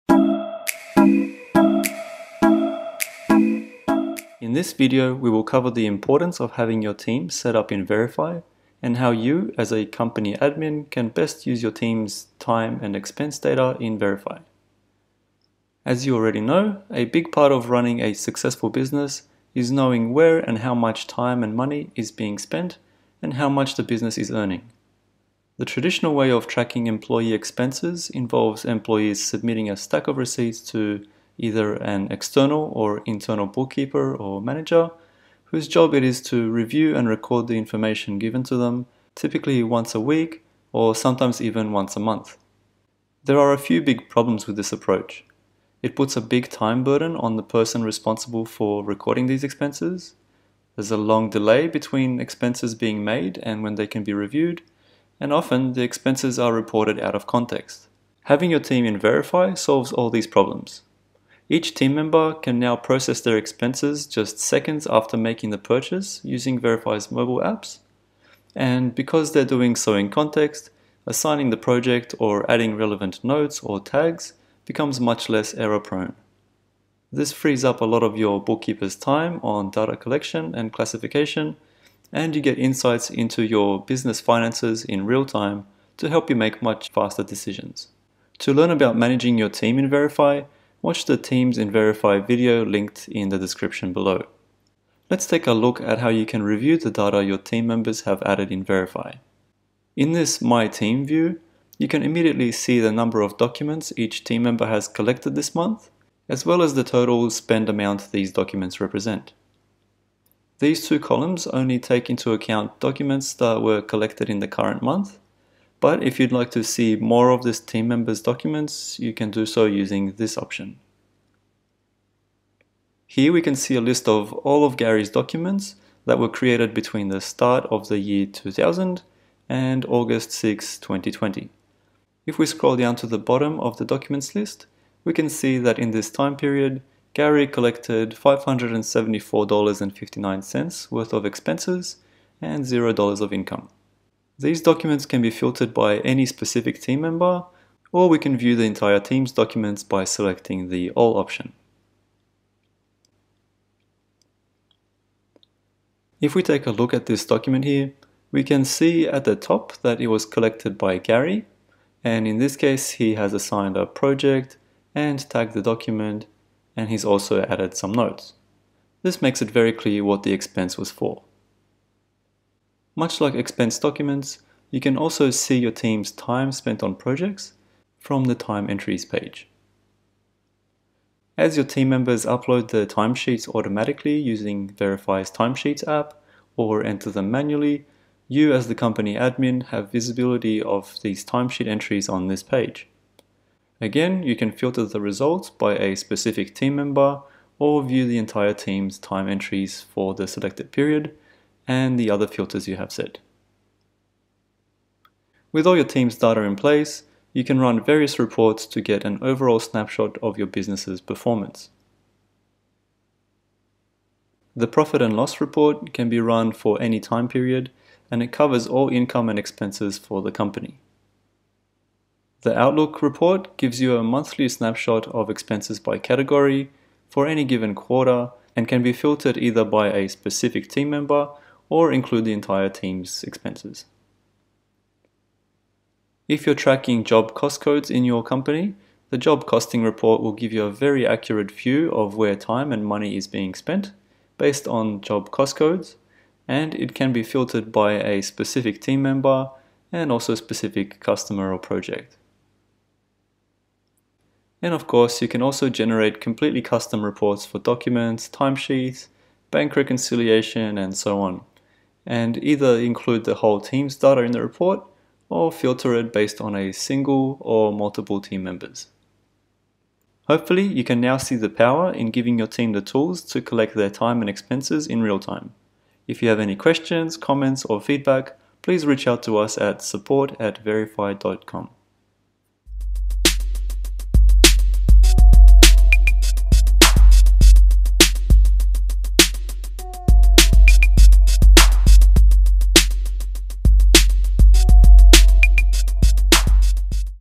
Workspace for Teams by Veryfi
Workspace for Teams brings spend transparency across your organization.

As a manager or owner, with Veryfi, you can now understand where and how much money is being spend and by who. Especially during Covid pandemic, being on top of your expenses is critical to business survival. Don't be that owner who didn't take action and regretted it.

The traditional means of keeping track of company expenses is to send them to a single entry point, often a finance controller. In smaller organizations it might be the owner. This is;
- Time burden on everyones time doing mundane tasks that should be automated,
- It creates long delay between expense reviews and
- Expenses are reported out of context so additional time is required chasing employees to understand what where and how.

With Veryfi you can fix all that;
- Automate by removing the manual data entry burden freeing your finance teams,
- Receive expenses in real-time to get insights into your business activity and
- Understand the why and how to your company's expenses.